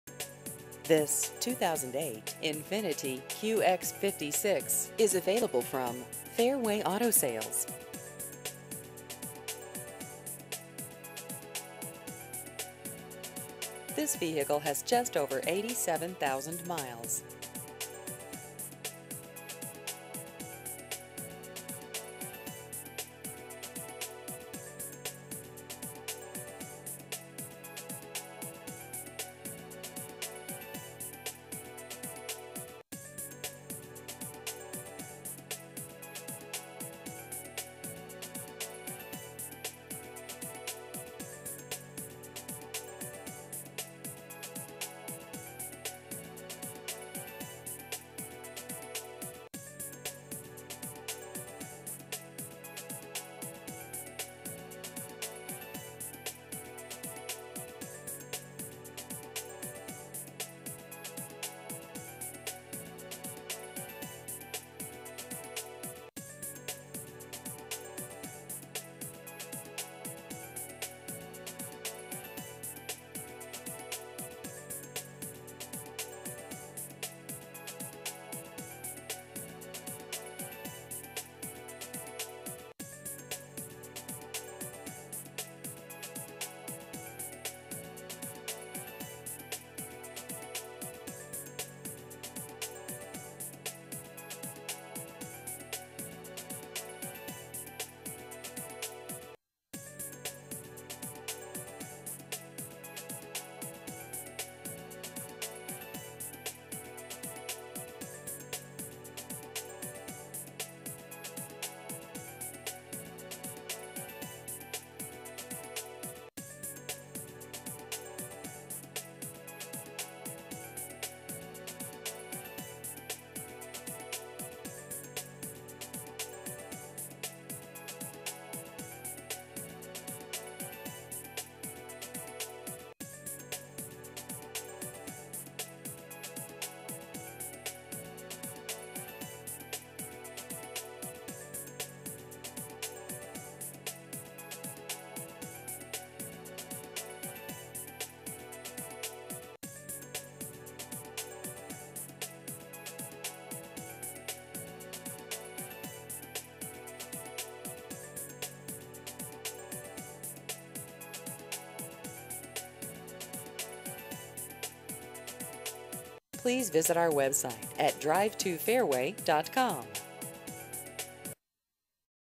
2008 Infiniti QX56 Used Cars Salt Lake City UT
This 2008 Infiniti QX56 is available from Fairway Auto Sales.